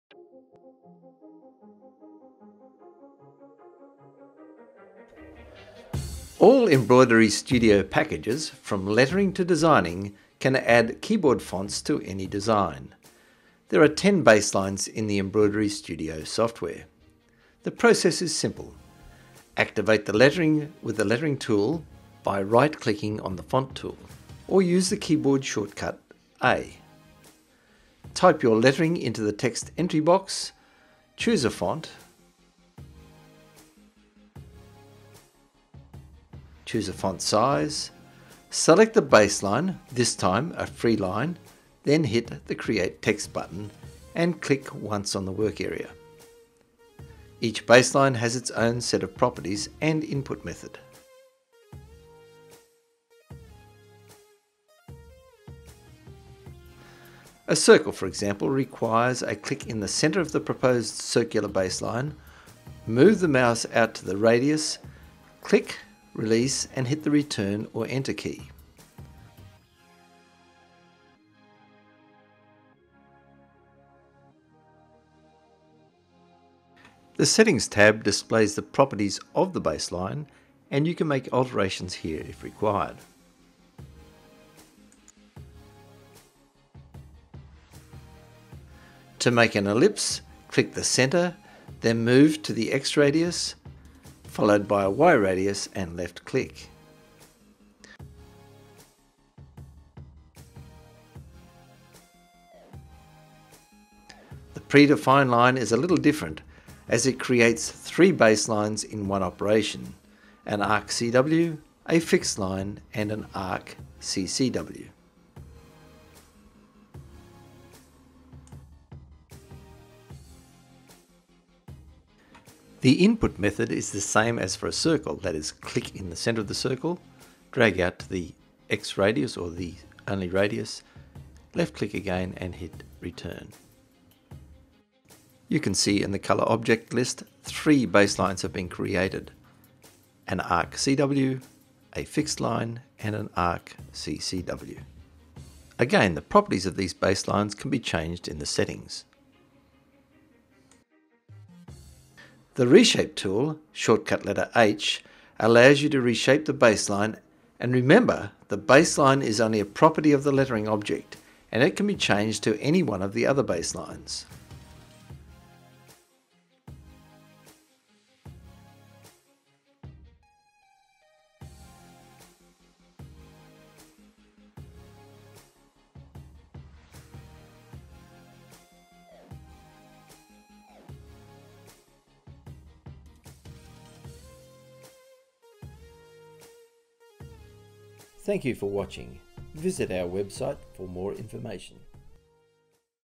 Add lettering to any design in Wilcom's EmbroideryStudio e4
John explains lettering in Wilcom's EmbroideryStudio e4, including baseline, fonts, size and sequence.

Printed copies of the EmbroideryStudio User Guide can be ordered from Amazon.com

# embroiderylettering